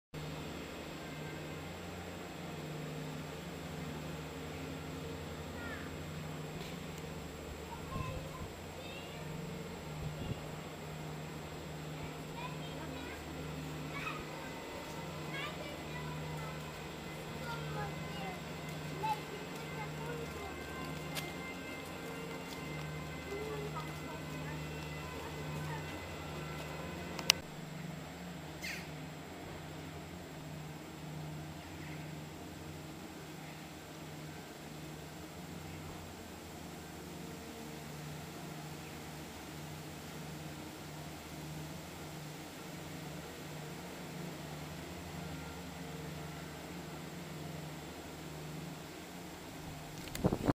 Following a butterfly
Just a short clip of a butterfly :D
Sorry it's shaky, I need to get a tripod...